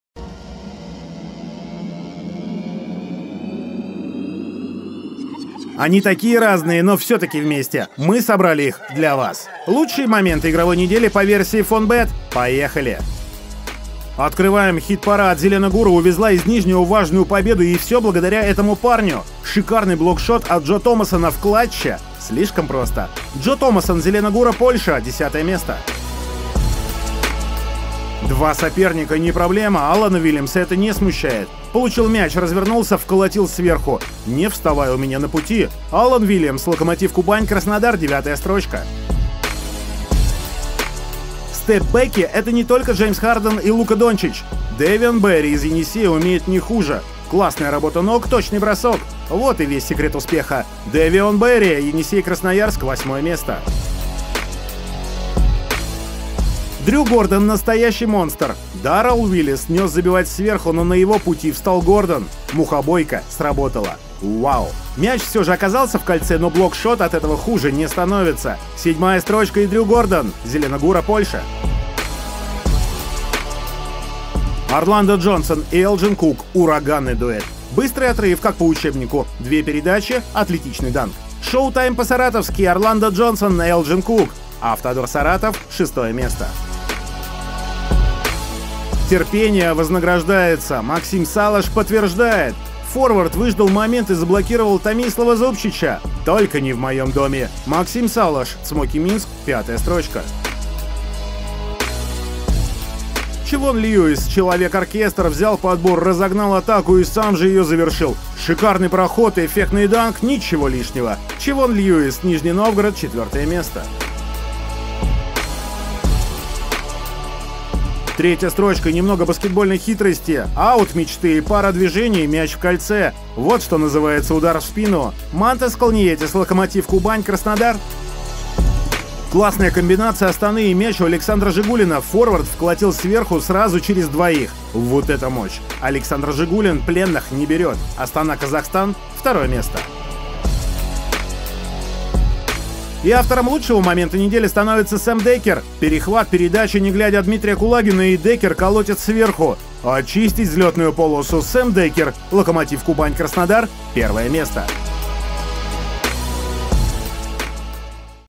Week 8. Top 10 Plays | Season 2019/20
🎬 Check out Top-10 highlights of the Week #8 featuring Sam Dekker, Mantas Kalnietis, Chavaughn Lewis and more!